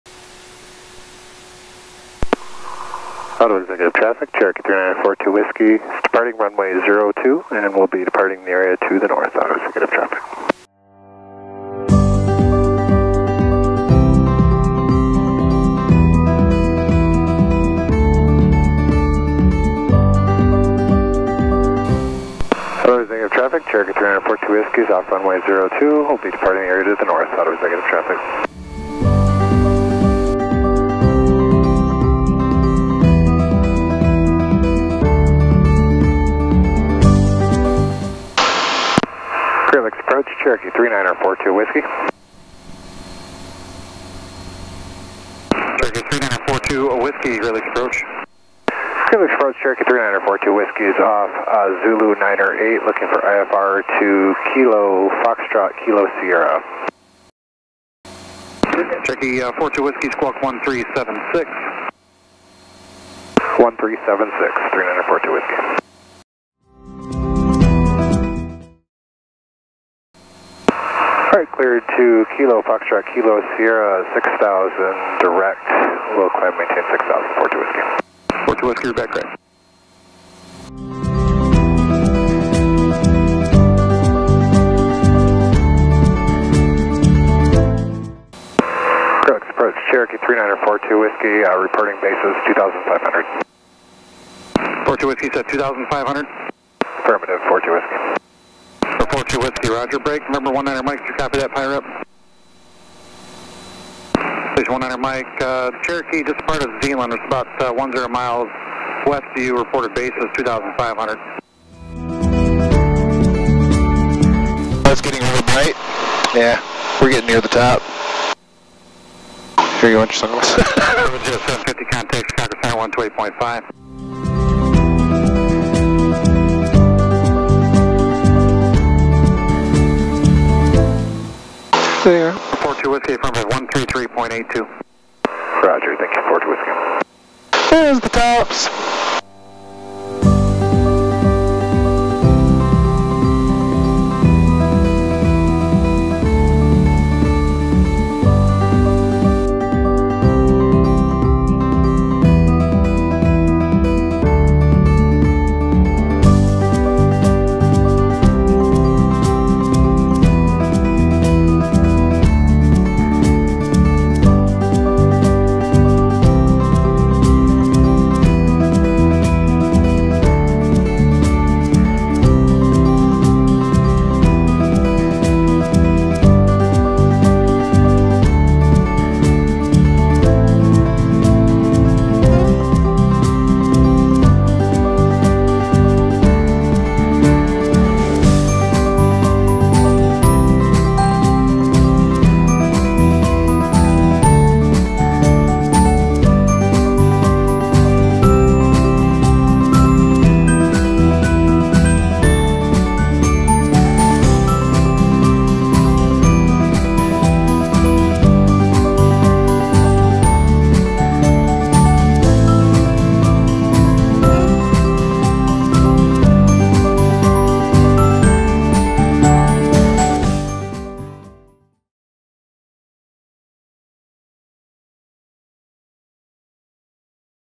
Cloudy Labor Day......Let's go find some sun. PART 1
We were a bit bummed by the gloomy day on Labor day so Michele and I went and did something about it.  Short flight up to Frankfort to find some sun.  Part 2 we'll check out the Sleeping Bear Dunes from the air!